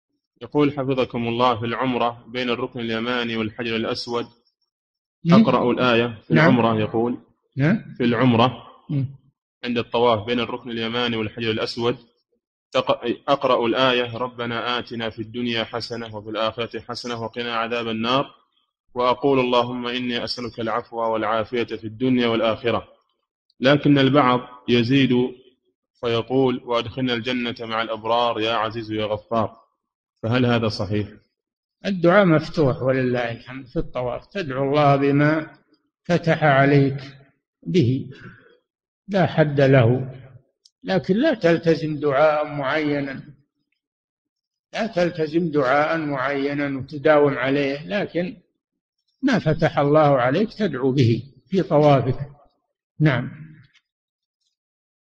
[357 -521] هل يجوز للمعتمر أن يزيد في الدعاء الوارد بين الركن اليماني والحجر الأسود؟ - صالح الفوزان
-[357 -521] السؤال: في العمرة بين الركن اليماني والحجر الأسود أقرأ الآية: ﴿ رَبَّنَا آتِنَا فِي الدُّنْيَا حَسَنَةً وَفِي الآخِرَةِ حَسَنَةً وَقِنَا عَذَابَ النَّارِ ﴾[البقرة:201]، وأقول: اللهم إني أسألك العفو والعافية في الدنيا والآخرة، لكن البعض يزيدُ فيقول: وأدخلنا الجنة مع الأبرار، يا عزيز يا غفَّار، فهل هذا صحيح؟
الجواب: الدعاء مفتوح –ولله الحمد- في الطواف، تدعو الله بما فتح عليك به، لا حدَّ له، لكن لا تلتزم دعاءً مُعينًا، وتداوم عليه، لكن ما فتح الله عليك تدعو به في طوافك.
للاشتراك في قناة الشيخ صالح الفوزان

للاستفسار والإقتراحات والدعم
رئيس الفريق
لمشروع كبار العلماء